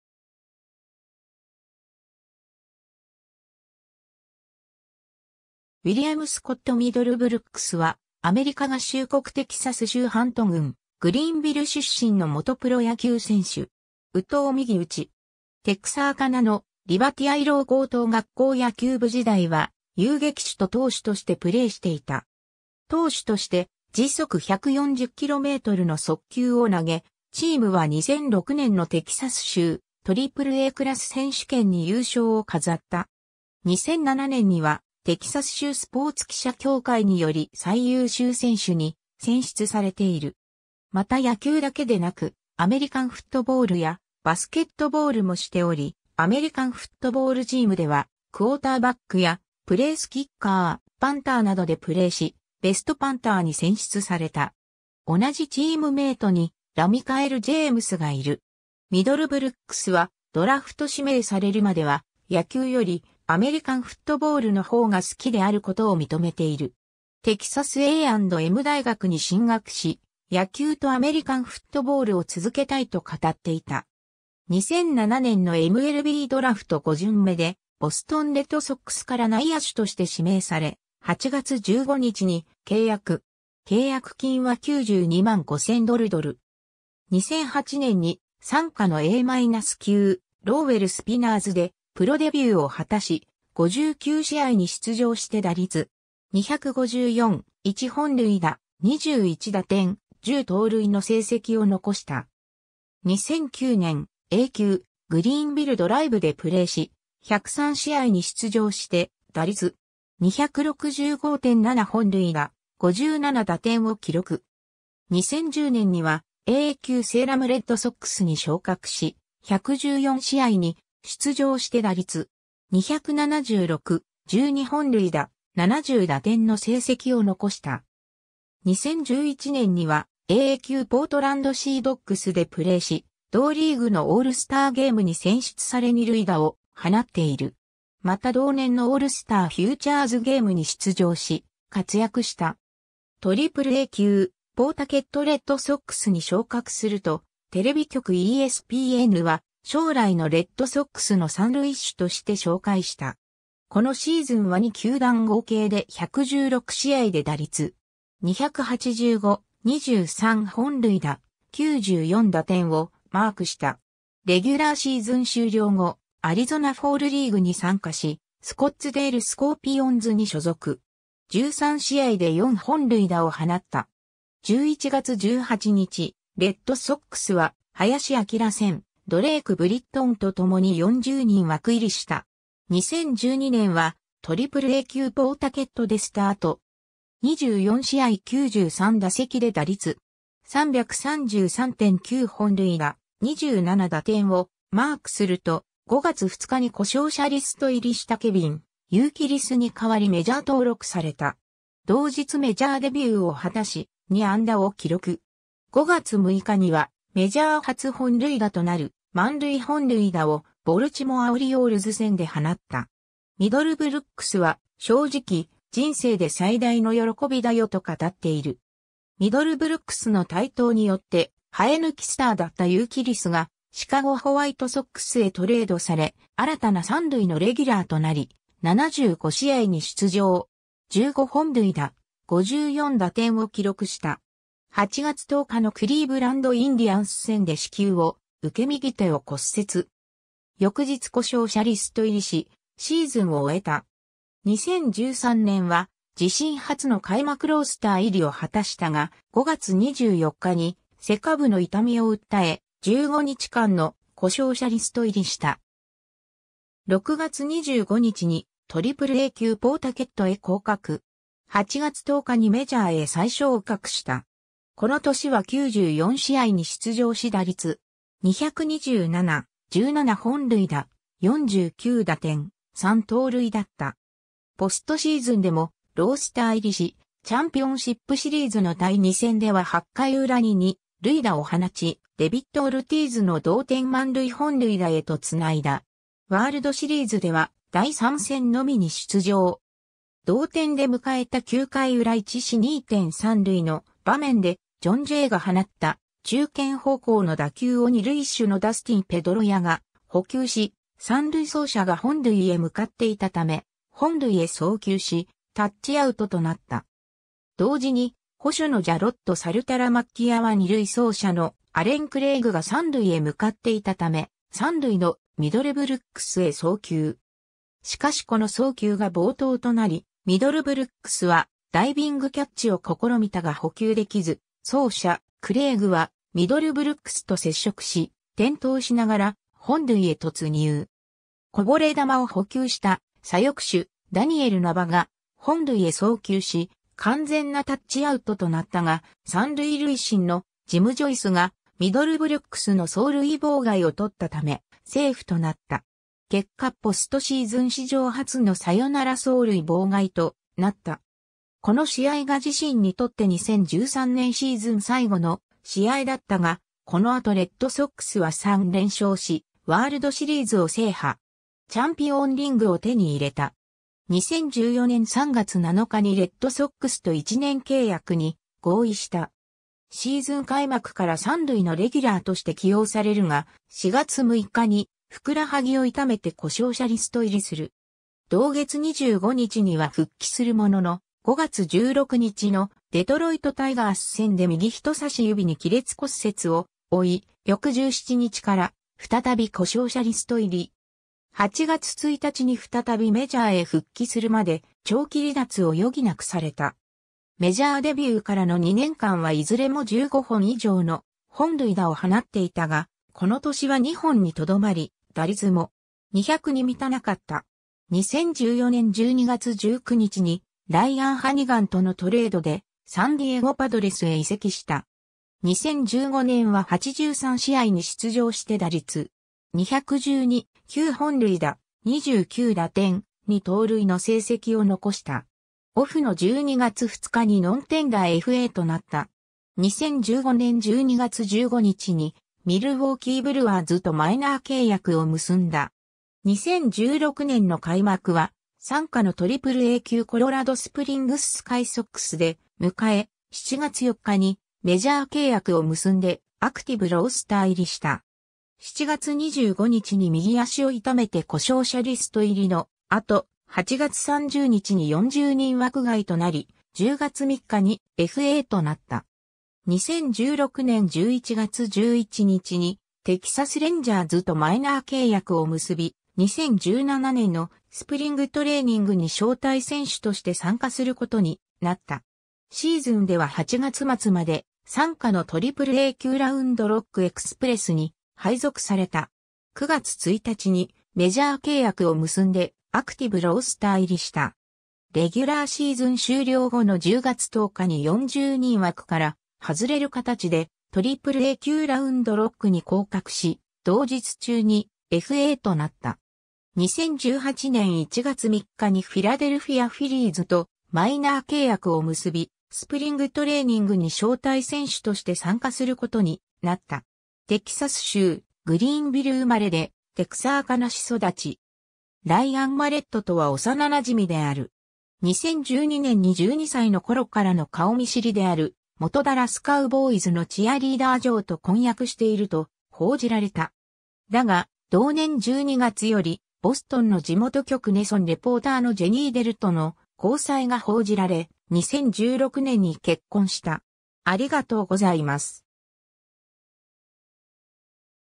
ウィル・ミドルブルックス
ウィリアム・スコット・ミドルブルックス（William "Will" Scott Middlebrooks , 1988年9月9日 - ）はアメリカ合衆国テキサス州ハント郡グリーンビル出身の元プロ野球選手（三塁手）。
右投右打。
 テクサーカナのリバティ・アイロウ高等学校野球部時代は、遊撃手と投手としてプレーしていた。
投手として、140km/hの速球を投げ、チームは2006年のテキサス州AAAクラス選手権に優勝を飾った。
2007年には、テキサス州スポーツ記者協会により最優秀選手に選出されている 。
 また野球だけでなく、アメリカンフットボールやバスケットボールもしており、アメリカンフットボールチームでは、クォーターバックやプレースキッカー、パンターなどでプレーし、ベストパンターに選出された。
同じチームメイトにラミカエル・ジェームスがいる。
 ミドルブルックスはドラフト指名されるまでは野球よりアメリカンフットボールの方が好きであることを認めている。
テキサスA&M大学に進学し、野球とアメリカンフットボールを続けたいと語っていた。
 2007年のMLBドラフト5巡目（全体174位）でボストン・レッドソックスから内野手として指名され、8月15日に契約。
契約金は$925,000ドル（約7,200万円）。
 2008年に傘下のA-級ローウェル・スピナーズ（ニューヨーク・ペンリーグ）でプロデビューを果たし、59試合に出場して打率.254、1本塁打、21打点、10盗塁の成績を残した。
 2009年、A級グリーンビル・ドライブ（サウス・アトランティックリーグ）でプレーし、103試合に出場して打率.265・7本塁打・57打点を記録。
2010年にはA+級セイラム・レッドソックス（カロライナリーグ）に昇格し、114試合に出場して打率.276、12本塁打、70打点の成績を残した。
 2011年にはAA級ポートランド・シードッグス（イースタンリーグ）でプレーし、同リーグのオールスターゲームに選出され二塁打を放っている。
また同年のオールスター・フューチャーズゲームに出場し、活躍した。
AAA級ポータケット・レッドソックスに昇格すると、テレビ局ESPNは「将来のレッドソックスの三塁手」として紹介した。
このシーズンは2球団合計で116試合で打率.285、23本塁打、94打点をマークした。
レギュラーシーズン終了後、アリゾナ・フォールリーグに参加しスコッツデール・スコーピオンズに所属。
13試合で4本塁打を放った。
11月18日、レッドソックスは林哲セン、ドレイク・ブリットンと共に40人枠入りした。
 2012年はAAA級ポータケットでスタート。
24試合93打席で打率.333・9本塁打・27打点をマークすると、5月2日に故障者リスト入りしたケビン・ユーキリスに代わりメジャー登録された。
同日メジャーデビューを果たし、2安打を記録。
5月6日にはメジャー初本塁打となる満塁本塁打をボルチモア・オリオールズ戦で放った。
ミドルブルックスは「正直、人生で最大の喜びだよ」と語っている。
ミドルブルックスの台頭によって生え抜きスターだったユーキリスがシカゴ・ホワイトソックスへトレードされ、新たな三塁のレギュラーとなり、75試合に出場。
15本塁打・54打点を記録した。
8月10日のクリーブランド・インディアンス戦で死球を受け右手を骨折。
翌日故障者リスト入りし、シーズンを終えた。
 2013年は自身初の開幕ロースター入りを果たしたが、5月24日に背下部の痛みを訴え、15日間の故障者リスト入りした。
6月25日にAAA級ポータケットへ降格。
8月10日にメジャーへ再昇格した。
この年は94試合に出場し打率.227、17本塁打、49打点、3盗塁だった。
ポストシーズンでもロースター入りし、チャンピオンシップシリーズの第2戦では8回裏に二塁打を放ち、デビッド・オルティーズの同点満塁本塁打へと繋いだ。
ワールドシリーズでは第3戦のみに出場。
同点で迎えた9回裏1死2・3塁の場面でジョン・ジェイが放った中堅方向の打球を二塁手のダスティン・ペドロイアが捕球し、三塁走者が本塁へ向かっていたため...